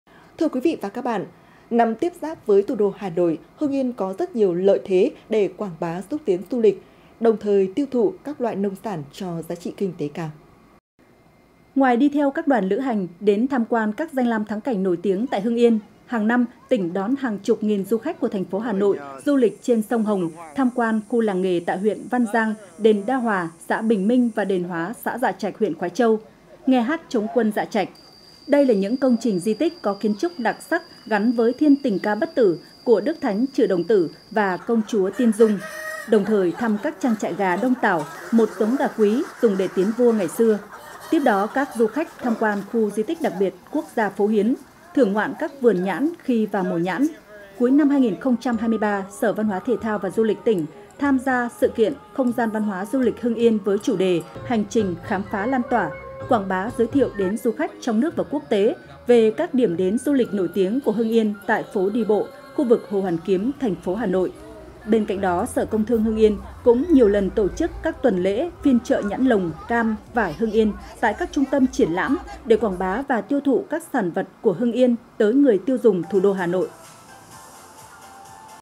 Quảng bá du lịch và nông sản Hưng Yên tại Thủ đô Hà Nội
Hưng Yên TV | Quảng bá du lịch và nông sản Hưng Yên tại Thủ đô Hà Nội

Tiktok: Truyền hình Hưng Yên
Facebook: Đài Phát thanh và Truyền hình Hưng Yên
Zalo: Đài PTTH Hưng Yên